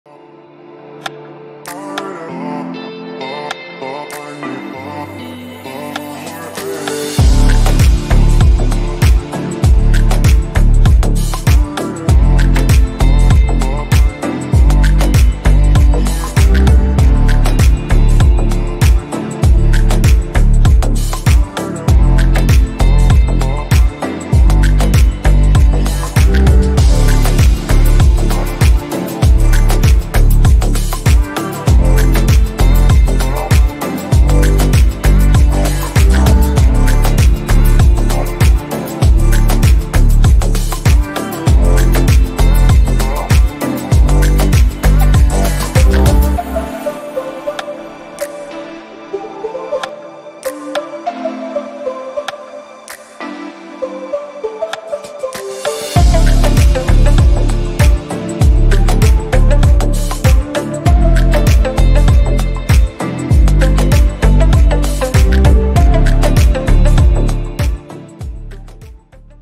Paseo Country Downtown
Recorrido virtual del nuevo desarrollo Paseo Country Downtown.